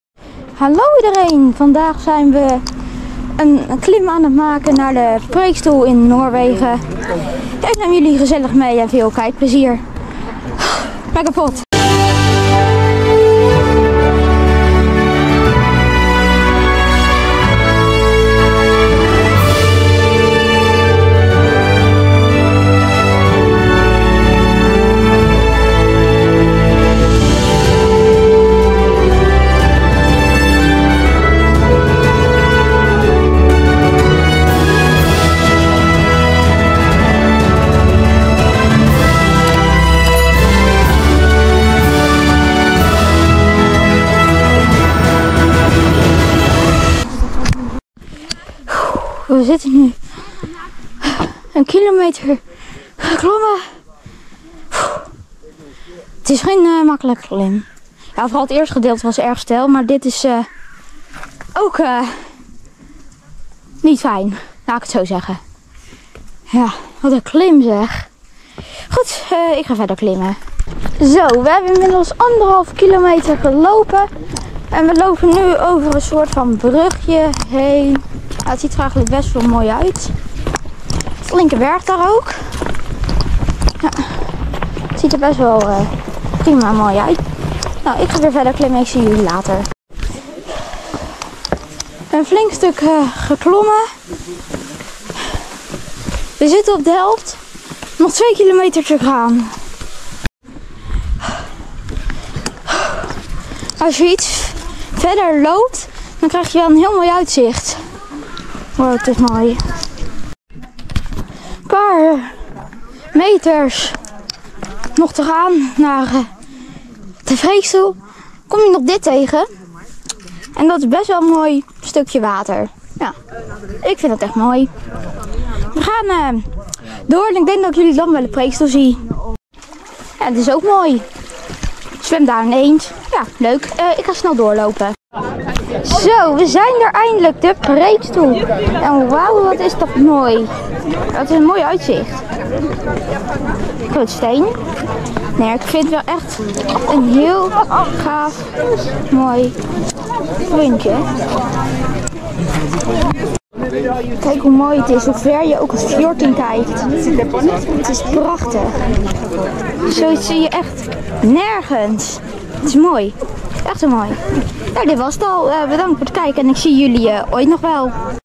preikenstolen beklimmen. (elmi's vakantie vlog 2024 #2)
deze video beklim ik de preikenstolen in het nederlands ook wel de preekstoel maar goed jullie kunnen geen
Noors!

henk de potvis